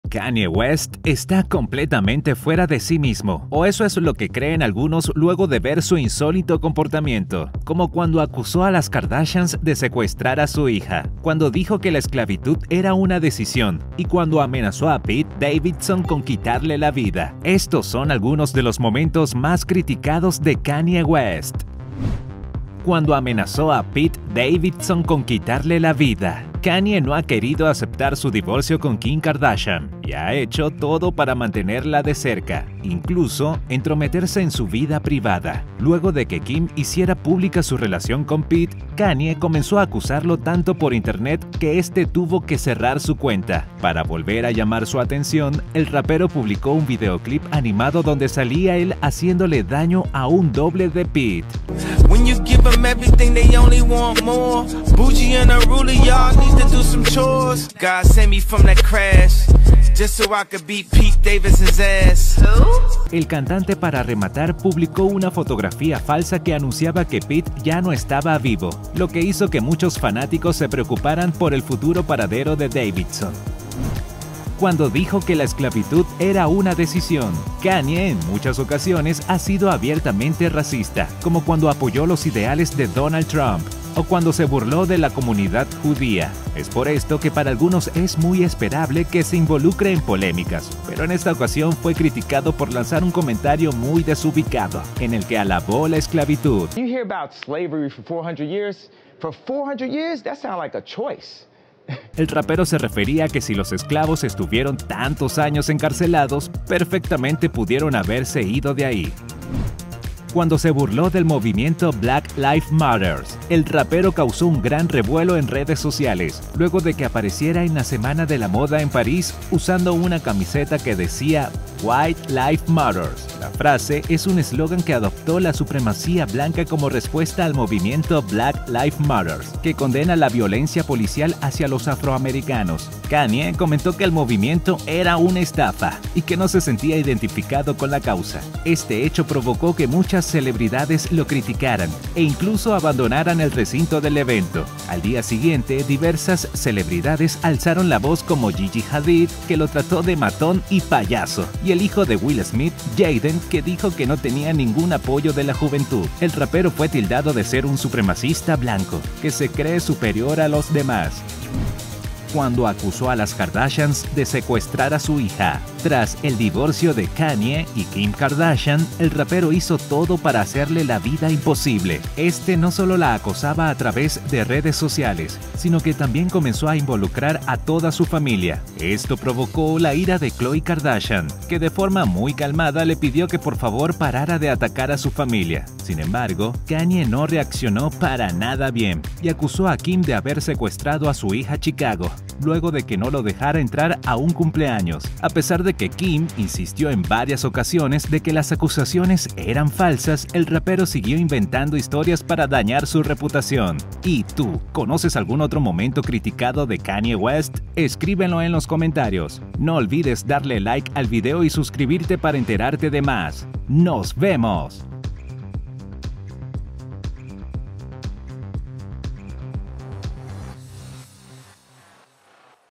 Los momentos más criticados de Kanye West | Showbiz
Kanye West está completamente fuera de sí mismo, o eso es lo que creen algunos luego de ver su insólito comportamiento. Como cuando acusó a las Kardashians de secuestrar a su hija, cuando dijo que la esclavitud era una "decisión", y cuando amenazó a Pete Davidson con quitarle la vida. Estos son algunos de los momentos  más criticados de Kanye West.

¿Te gustó nuestro canal? Cuéntanos sobre ti